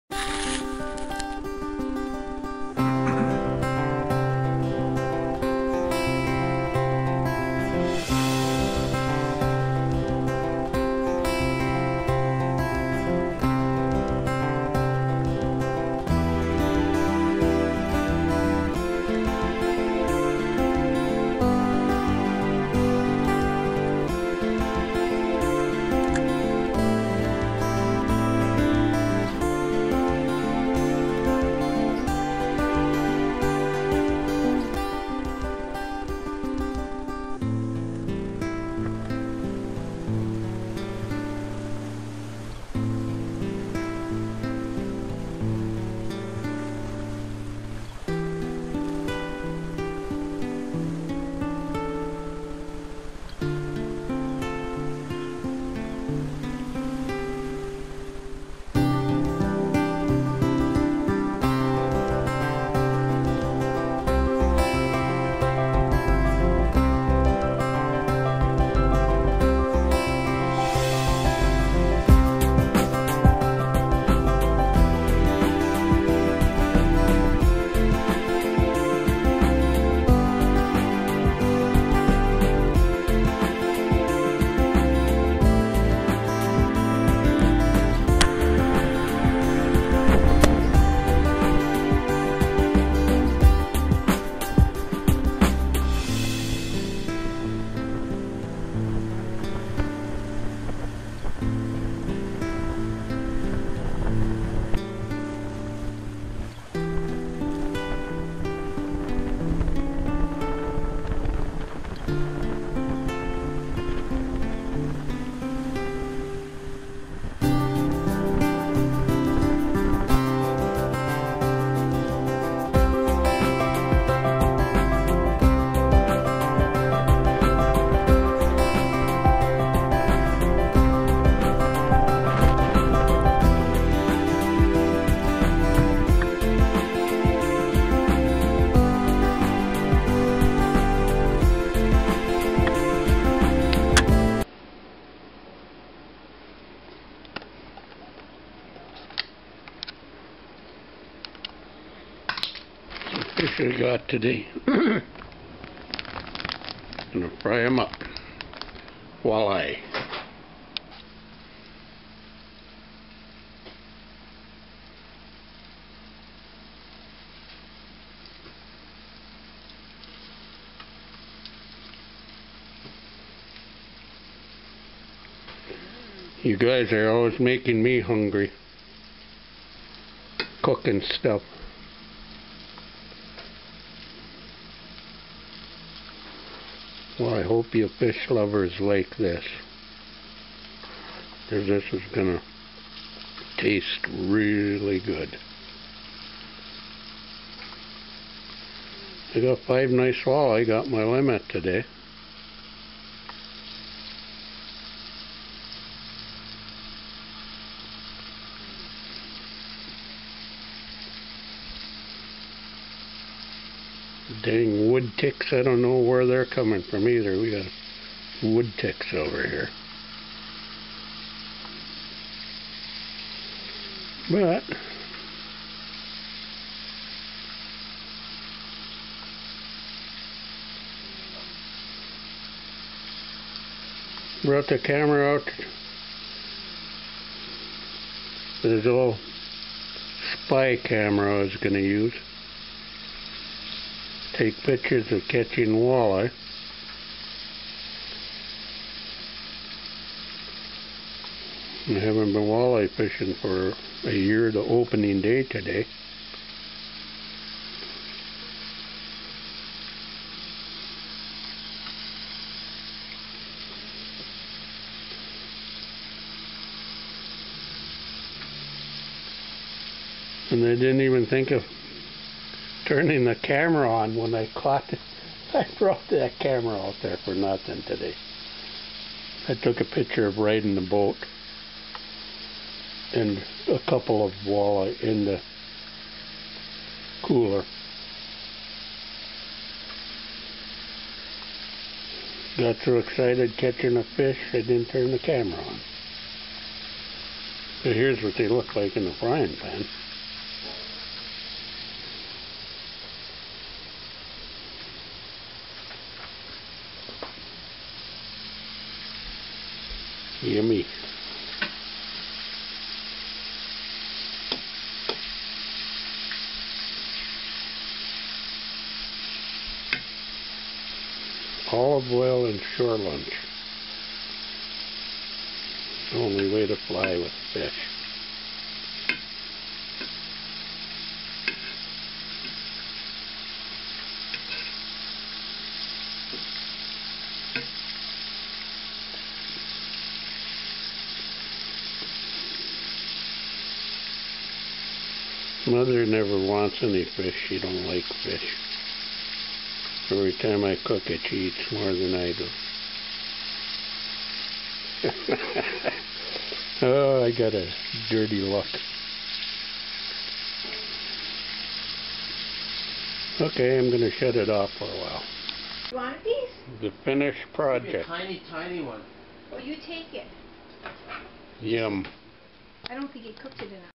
walleye opening day.
sorry for the china camera shakes.but did have good end results ill use my go pro next time.Huronbay .